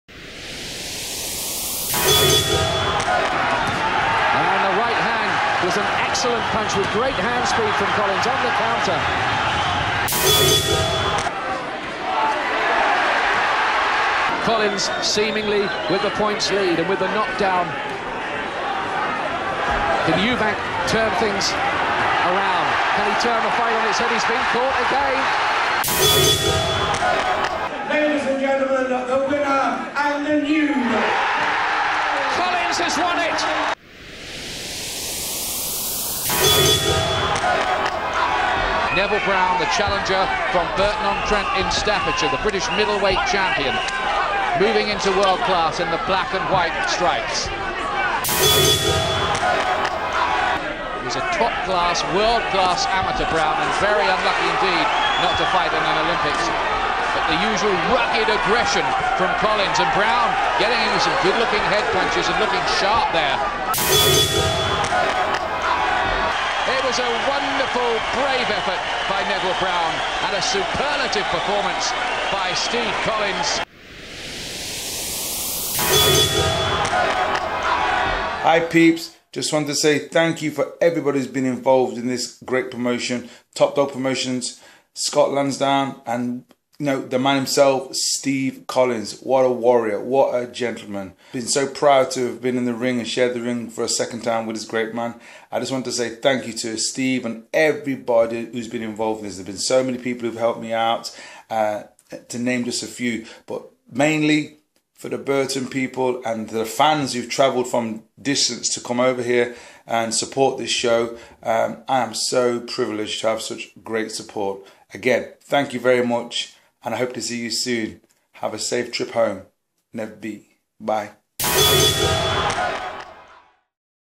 Neville Brown vs Steve Collins Trailer - 06/07/14
Top Dog Promotions presents Neville Brown vs Steve Collins, 6th July 2014